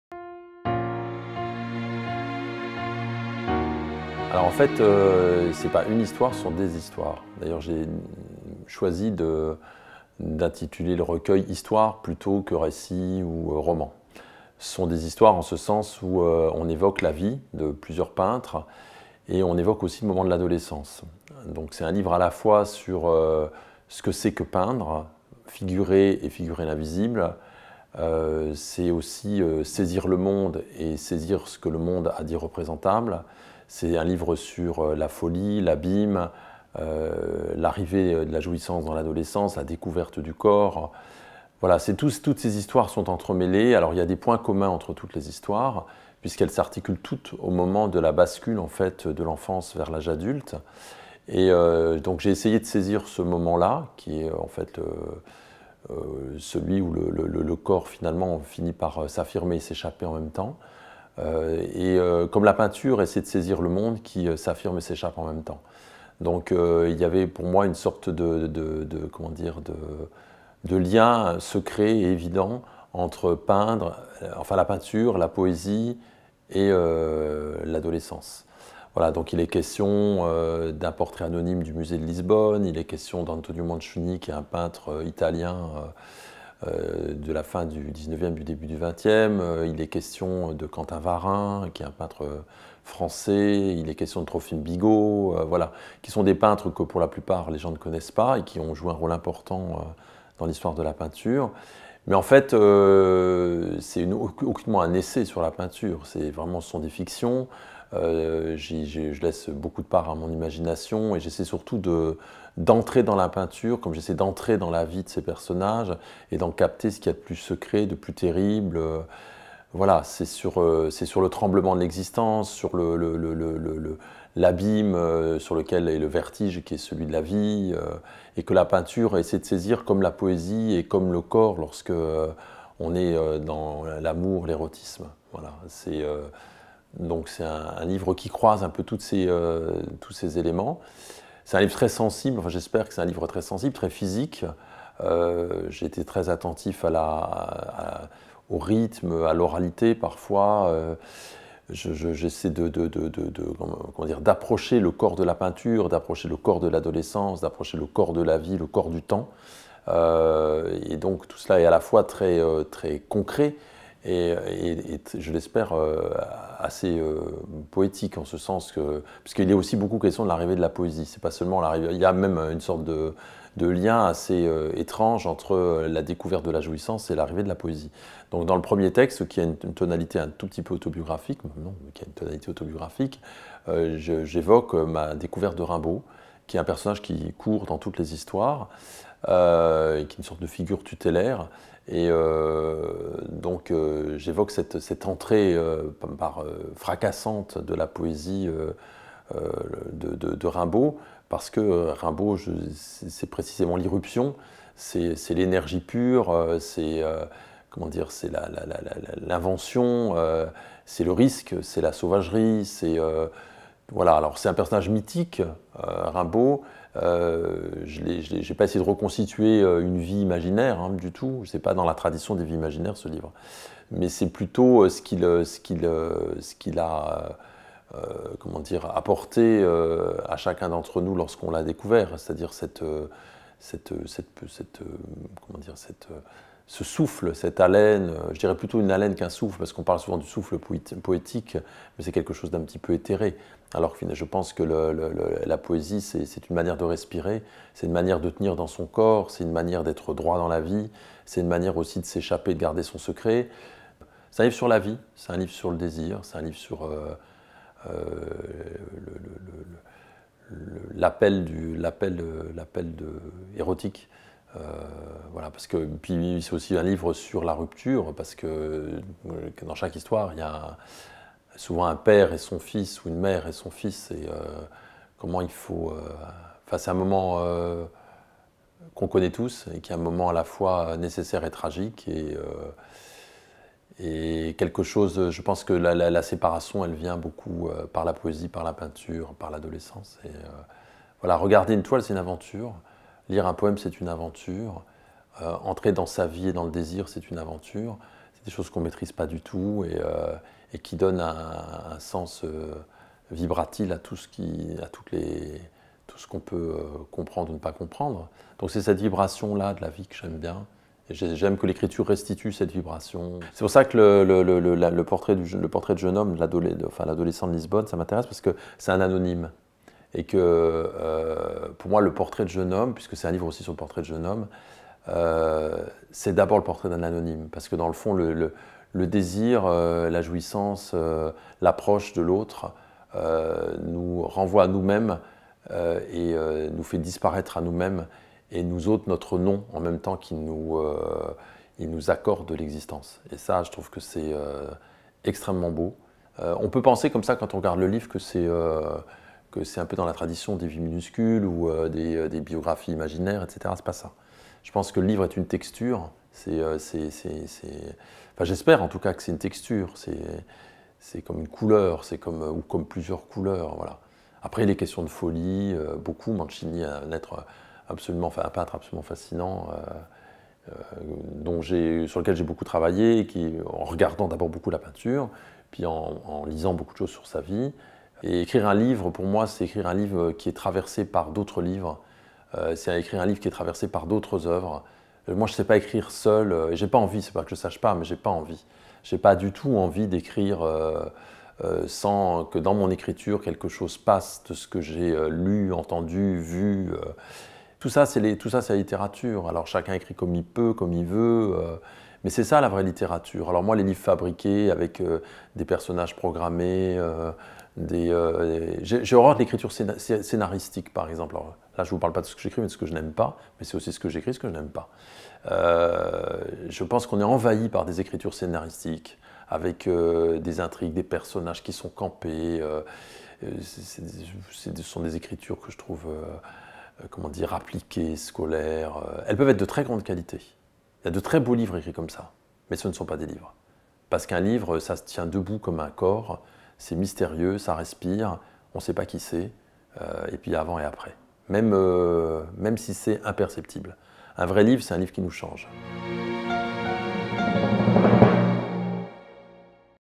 Guy Walter - Outre mesure, histoires
Guy Walter vous présente son ouvrage "Outre mesure, histoires" aux éditions Verdier. Rentrée littéraire mars 2014.